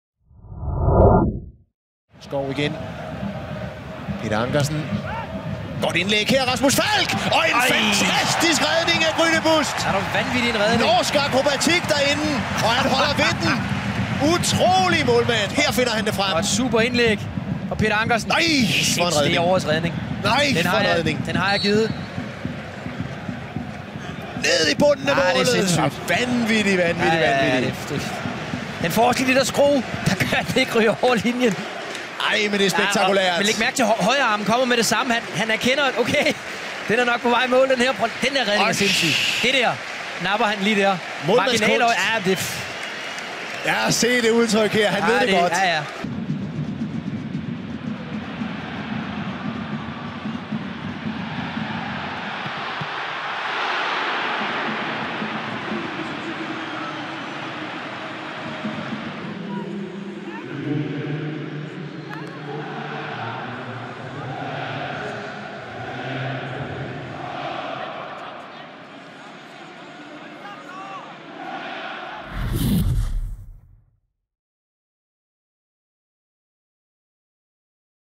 Ny FCK-keeper: Se Sten Grytebusts mirakelredninger mod sin kommende klub
Sten Grytebust tiltræder som ny FCK-keeper til sommer på en kontrakt frem til 31/12 2022. Tjek hans to fantastiske feberredninger i kampene mod os fra denne sæson lige her!

profilnavn: fc_kobenhavn